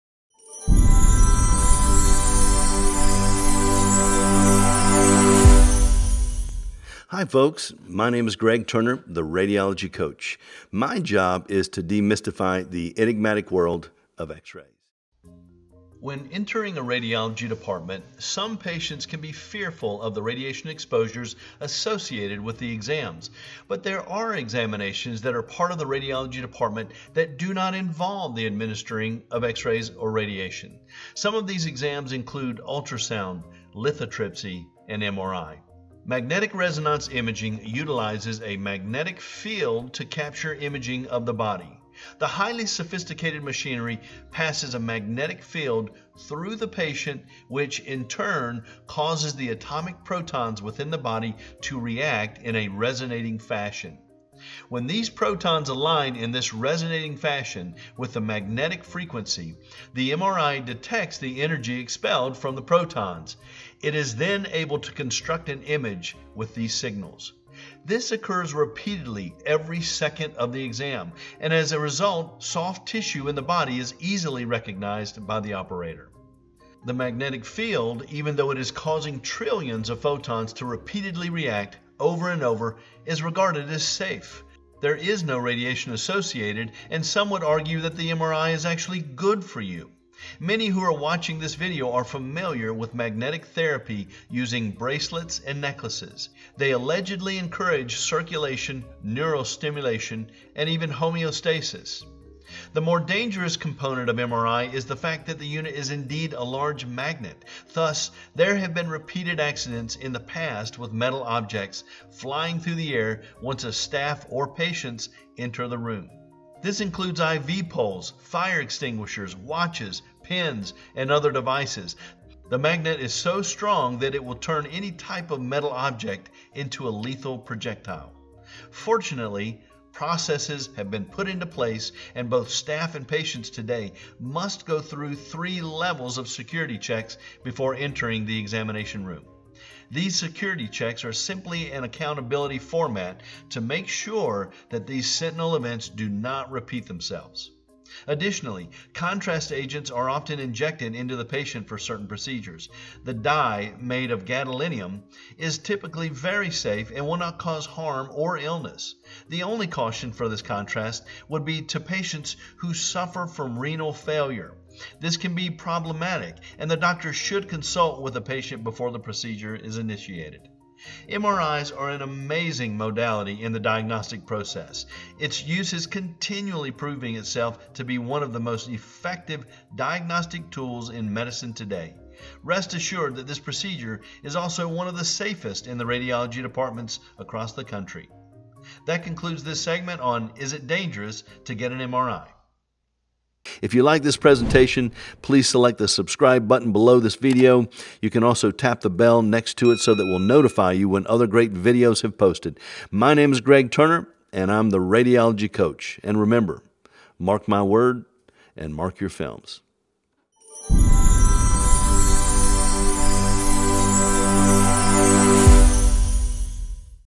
Is It DANGEROUS to Get an MRI??!!
MRI's are among the most mysterious of x-ray procedures. The principles involved with the imaging modality are hard to grasp for some. Here are some insights from The Radiology Coach on the effects, or lack thereof, of Magnetic Resonance Imaging.